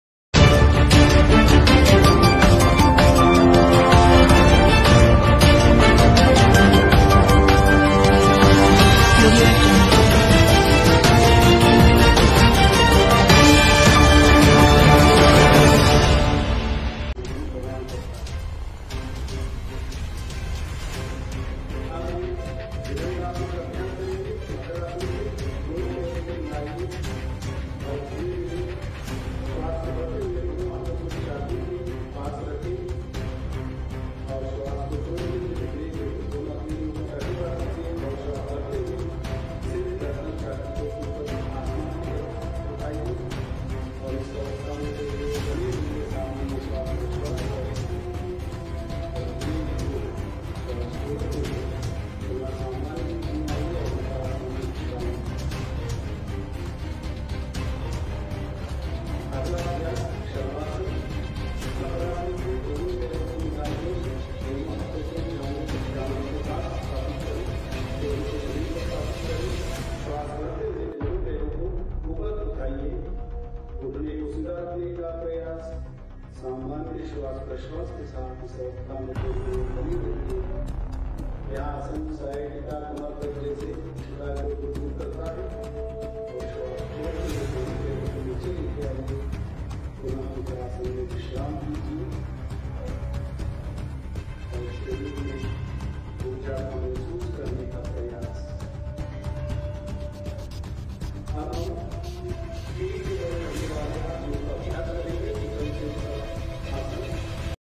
PM Modi participated in International Yoga Day 2024 celebrations at Dal Lake in Kashmir.
Glimpses of PM Narendra Modi’s interaction with the people of Srinagar after the yoga session